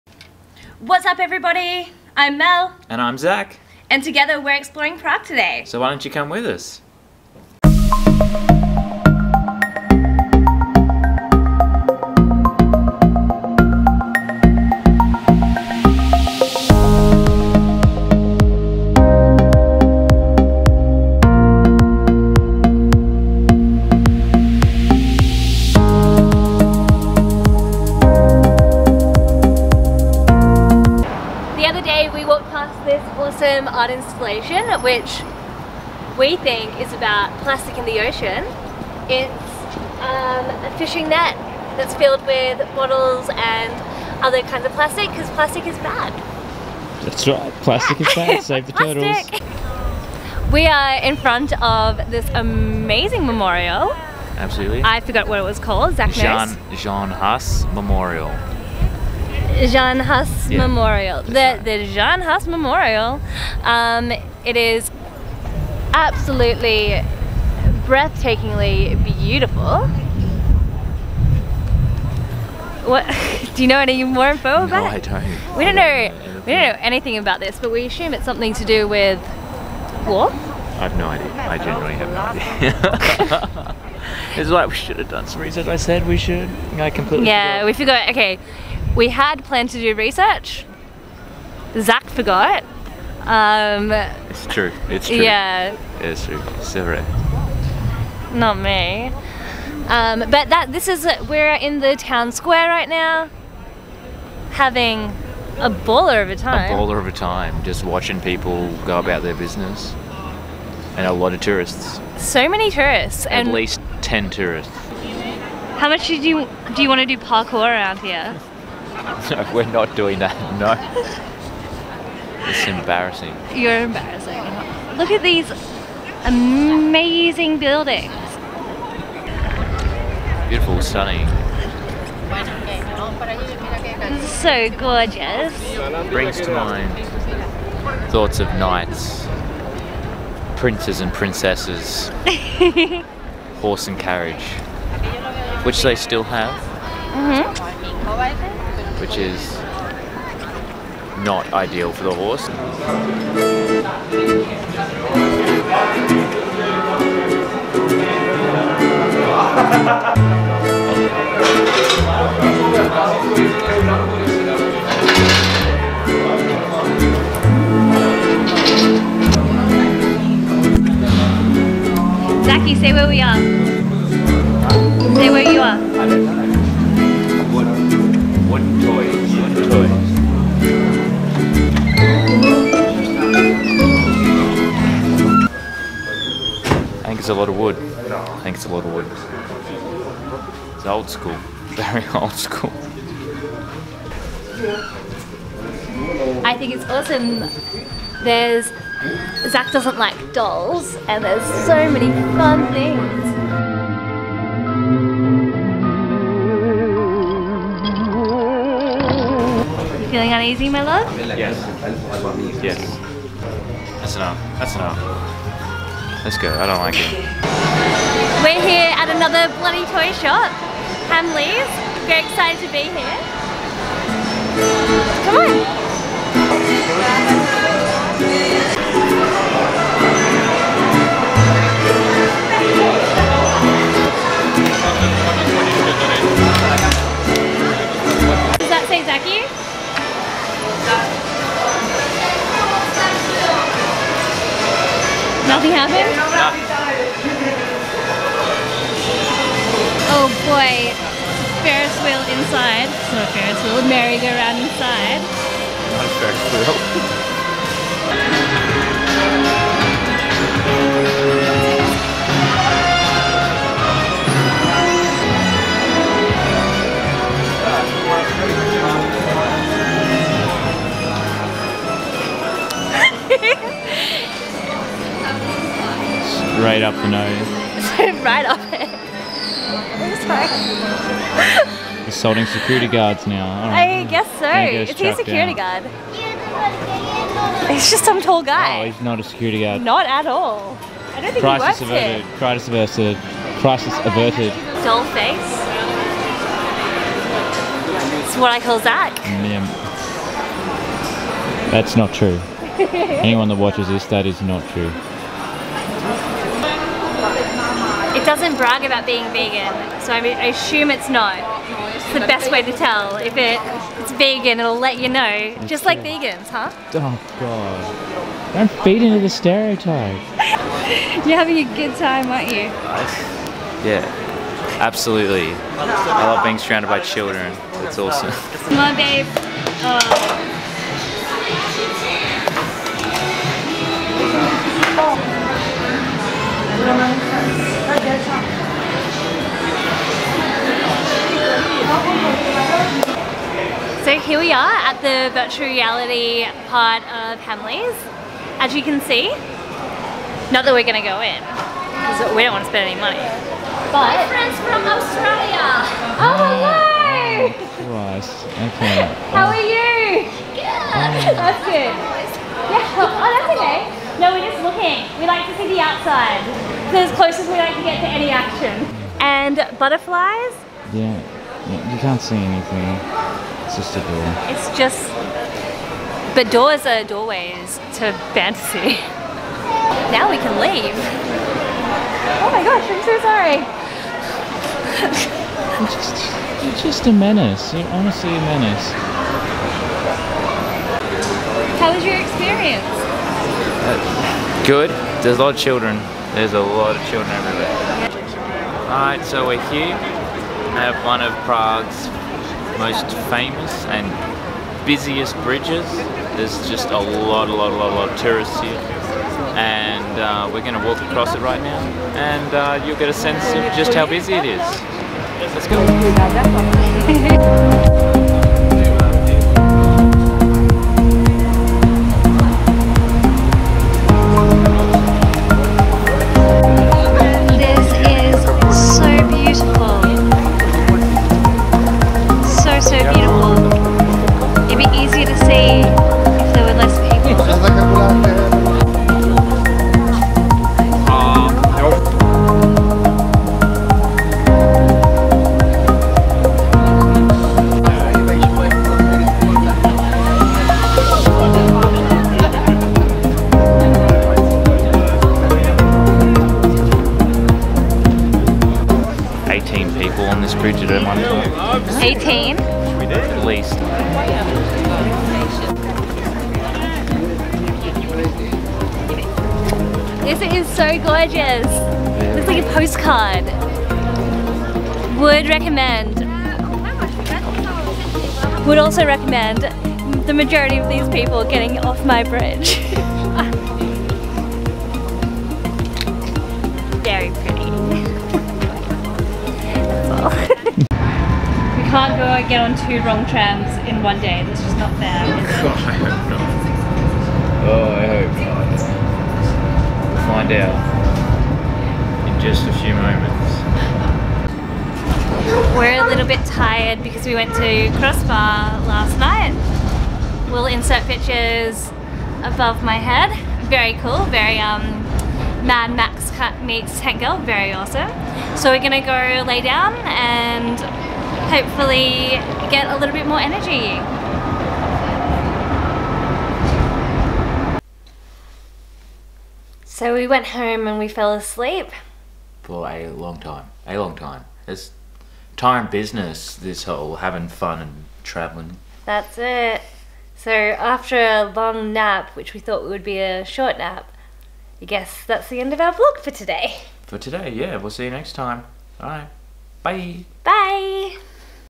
Travel Vlog || PRAGUE || Hamleys Toy Shop || Charles Bridge
Mel and Zac take on the fairytale that is Prague. Join us as we walk the streets and look at all that Prague has to offer.

Some of our top travel tips

Happy Cow- An app that shows you where to find everything vegan in the area you are travelling in.

Maps.ME- A app that shows you how to get around even when you are not connected to the internet.

For more of our wonderful life head to our instagram @melandzac